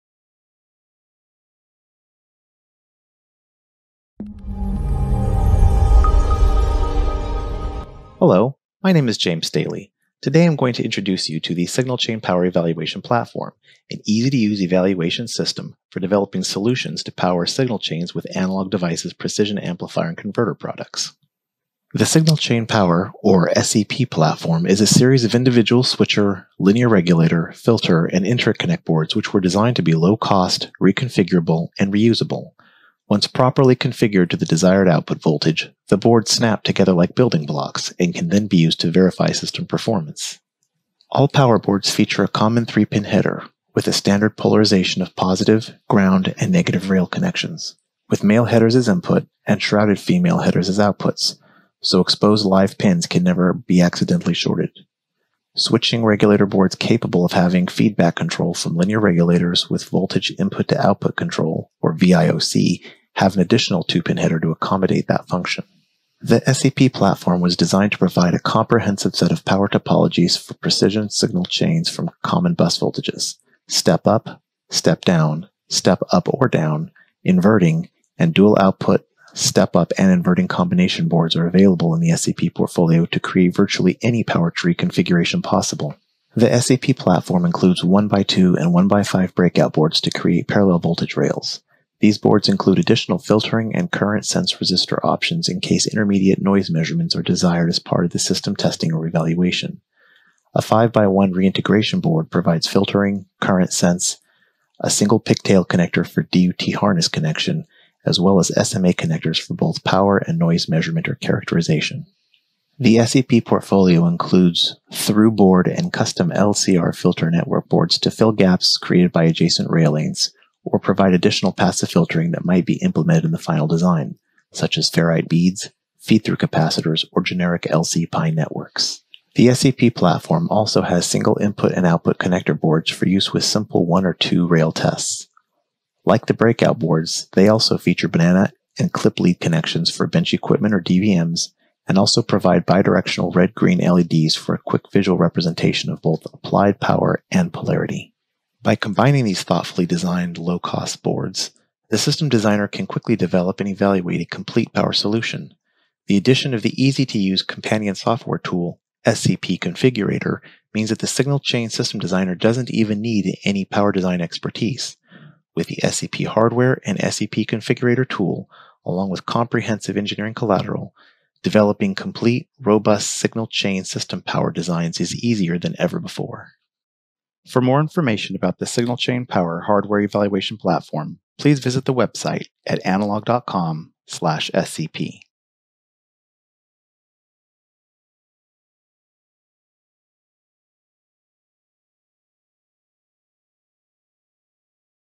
訊號鏈電源平台協助簡化評估流程
使用改進的標準展示板為儀器儀錶應用開發定製的電源樹需要大量時間。訊號鏈電源（SCP）是一款特殊的電源評估系統，將其組合在一起以實現快速雜訊性能。